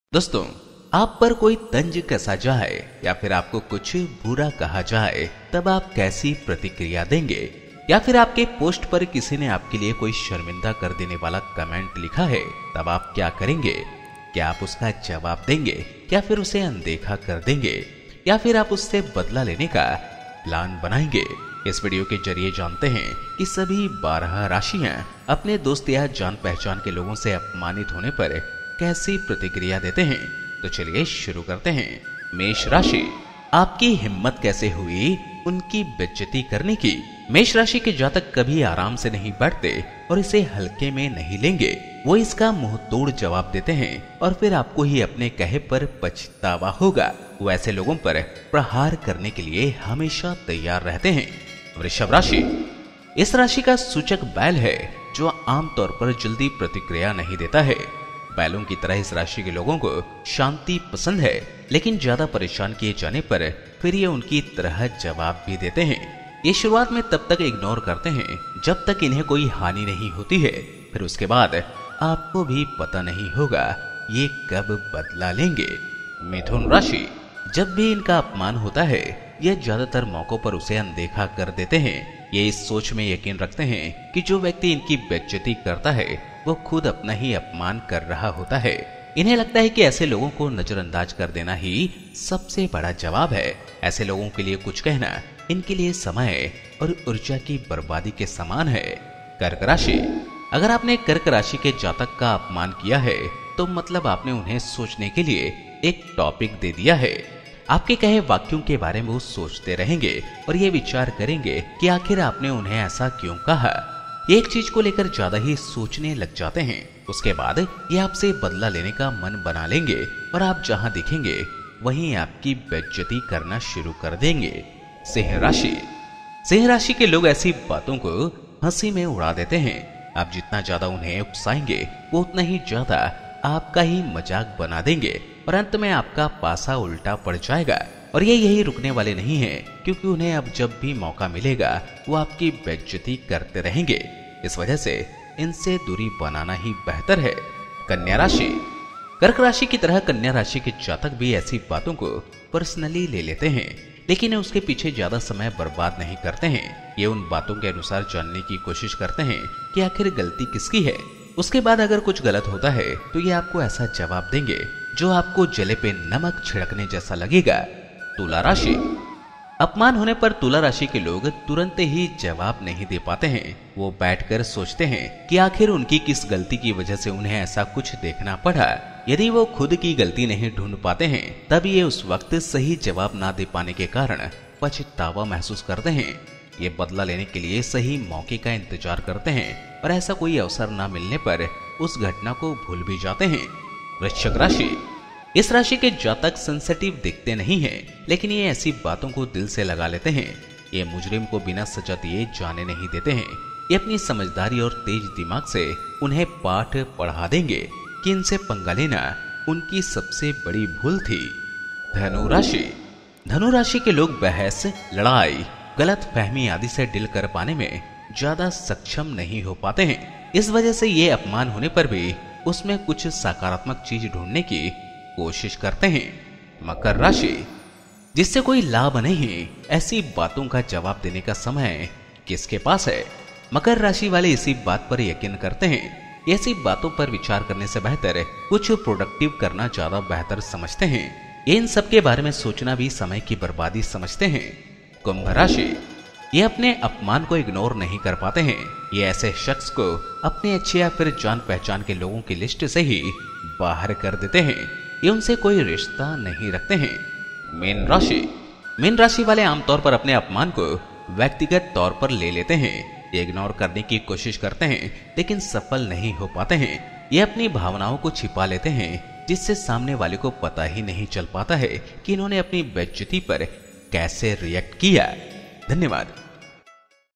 बेइज्जती पर चुप रहते हैं या लेते हैं बदला, राशि से जानें कैसे रिएक्ट करते हैं आप | Ghar Sansar Vastu
बेइज्जती पर चुप रहते हैं या लेते हैं बदला, राशि से जानें कैसे रिएक्ट करते हैं आप | Ghar Sansar Vastu Shastra
Your Reaction When You Are Insulted Based On Your Zodiac Sign
Here is how you would respond if you are insulted by somebody you really do not like. Five kinds of responses have been given below. Check out which category you fall into.